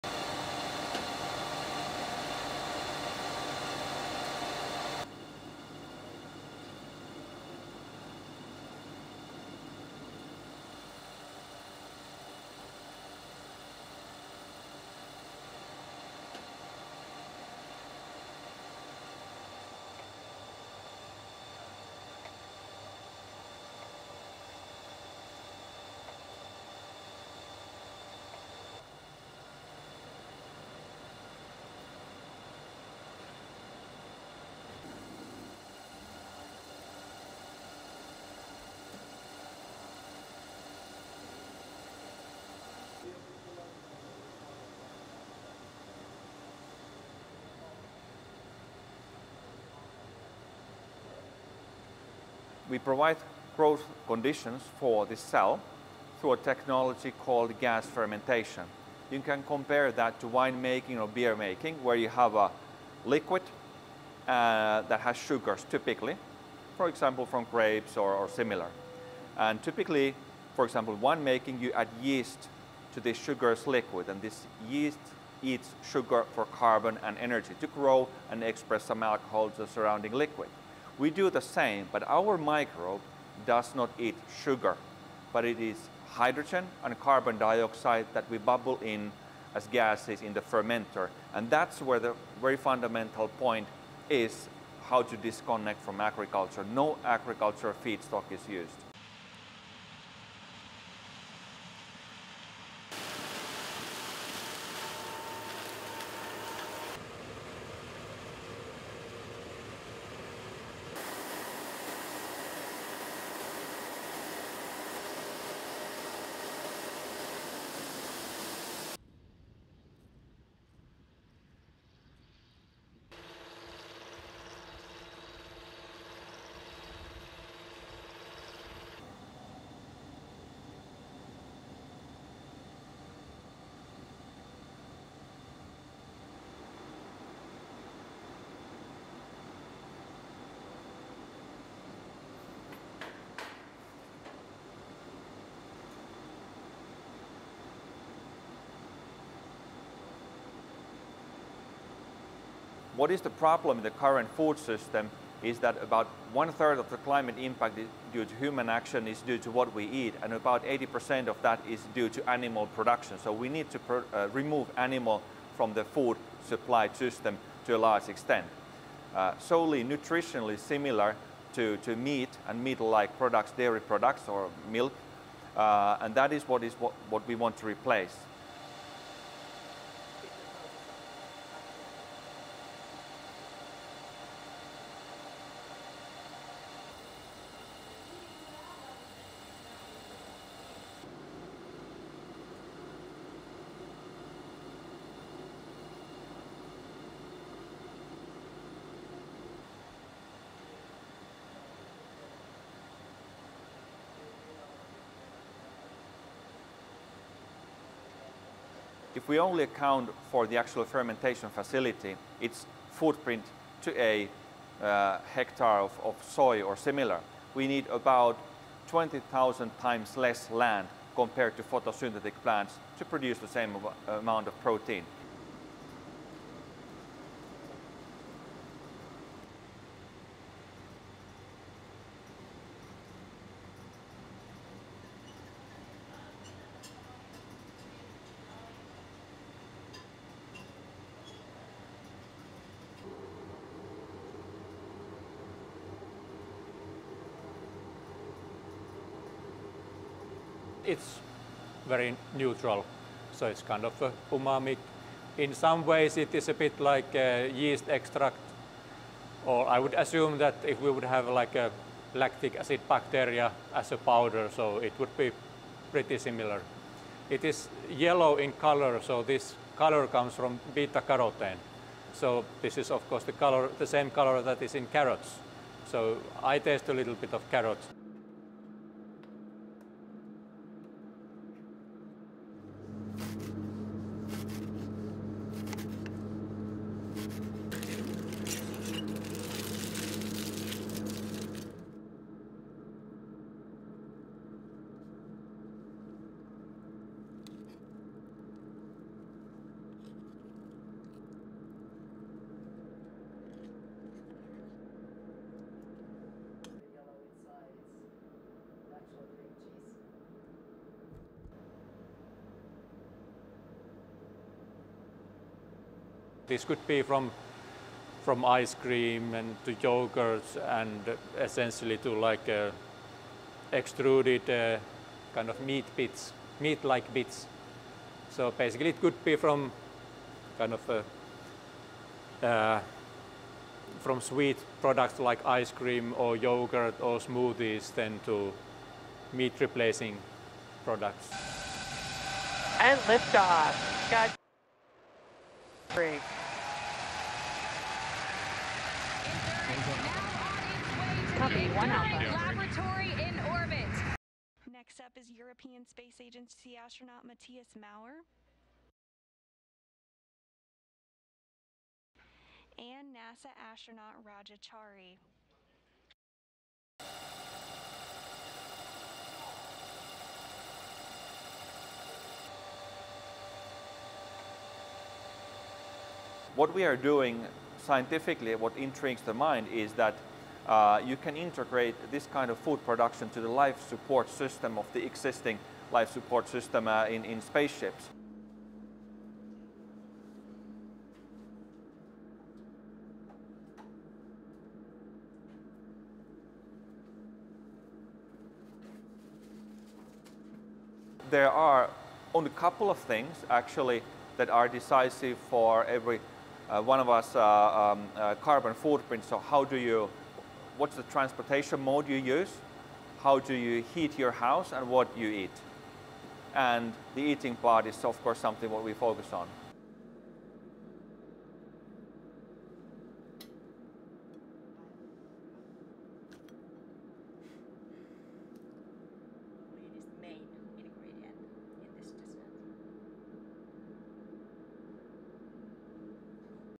Finnish food tech start-up makes protein from 'thin air'
(30 Dec 2021) LEAD IN:
A Finnish food tech start-up says it's making a new, nutrient-rich protein "from thin air".
Helsinki-based Solar Foods says its new protein can lessen the environmental impact of the world's food systems, which accounts for over a third of global greenhouse gas emissions.

STORY-LINE:
Could this be the farm of the future?
At this pilot plant on the outskirts of Helsinki, Finnish food tech start-up Solar Foods is making a protein "from thin air".
"Solein", as they call it, is grown in a fermentation tank from a single microbe.
The process uses carbon dioxide - from the air - and hydrogen that's split from water using electricity. A small amount of water - also from air - and minerals are also needed.
"We provide growth conditions for this cell, through a technology called gas fermentation. You can compare that to winemaking or beer making," explains Pasi Vainikka, the CEO of Solar Foods.
"Our microbe does not eat sugar, but it is hydrogen and carbon dioxide that we bubble in as gases in the fermenter."
According to a study published in Nature Food in March this year, the world's food systems are responsible for more than a third of global greenhouse gas emissions.
From land use change and agricultural production, to packaging and waste management, annual food system emissions were estimated to be 18 billion tonnes of carbon dioxide, about 34 percent of the global total.
And, according to the United Nations, food demand is expected to increase by between 60 and 100 percent, as Earth's population is predicted to rise to over 9.6 billion people by 2050.
Solar Foods is making food without these agricultural processes. When the start-up's electricity comes from renewable sources, there's almost zero carbon emissions.
The pilot plant is currently powered by hydro power, but they're seeking to use a mix of hydro, wind and solar.
The start-up was founded in 2017 as a spinoff from Finland's national research institute.
"What is the problem in the current food system is that about one third of the climate impact due to human action is due to what we eat, and about 80 percent of that is due to animal production," explains Vainikka.
"So, we need to remove animals from the food supply system to a large extent."
Indeed, the food tech start-up says its fermentation process is 20 times more efficient than photosynthesis - which helps grow crops in fields - and 200 times more than meat.
"If we only account for the actual fermentation facility, its footprint to a hectare of soy or similar, we need about 20,000 times less land compared to photosynthetic plants to produce the same amount of protein," says Vainikka.
After the micro organisms have grown and multiplied in water, they are then removed and dried.
And this is the result - a new protein, called "Solein". Like many other protein supplements, it has no strong, discernible taste.
"It's very neutral, so it's kind of a umami. In some ways, it is a bit like a yeast extract," explains Juha-Pekka Pitkanen, chief technology officer at Solar Foods.
"It is yellow in colour, so this colour comes from beta carotene. So, this is, of course, the same colour that is in carrots. So, I taste a little bit of carrot."
Here in this high-tech kitchen, Solar Foods product developers are creating potential dishes for the yellow-tinged protein powder. A recent, fun experiment saw them create these Solein cheesecake puffs, the main ingredient being a Solein cheese alternative.
"It could be from sweet products like ice cream or yoghurt or smoothies, then to meat replacing products," explains Pitkanen.